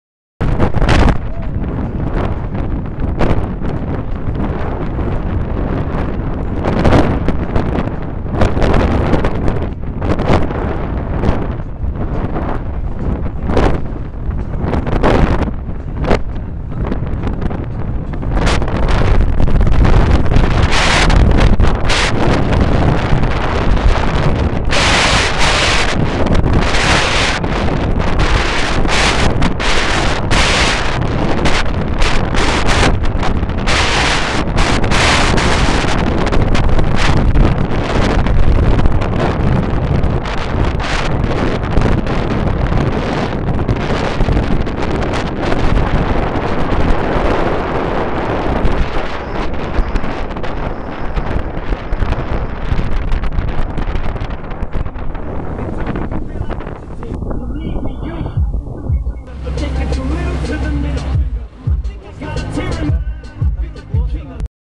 Swallows Nest Castle, Black Sea Speed Boat Trip, Ukraine / Russia
Black Sea Speed boat trip to the Swallows Nest Castle, Ukraine Russia